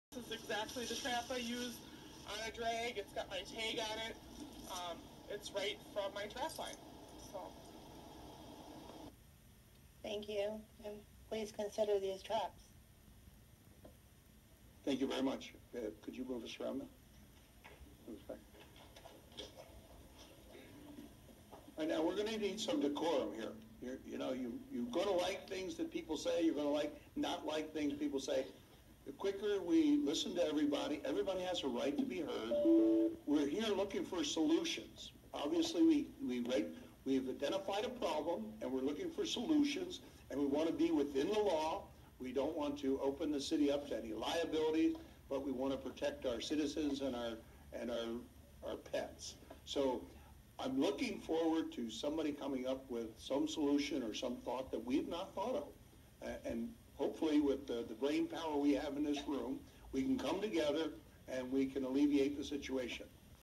Torrance, CA city council meeting clip
Earlier this week, the Torrance, California city council met to talk about the problem they have with urban coyotes killing so many pets in city residents’ backyards, and recently a toddler that was attacked by a coyote at the city park. My video was played to show that modern day traps aren’t what PETA and other anti-trapping groups tell you they are. The group Evict Coyotes worked so hard and they essentially “won”, by the city voting to hire trappers as soon as next fall to trap for half the year and try to manage the overpopulation and decrease depredation of beloved pets, and more importantly, opened their minds to using modified, humane traps.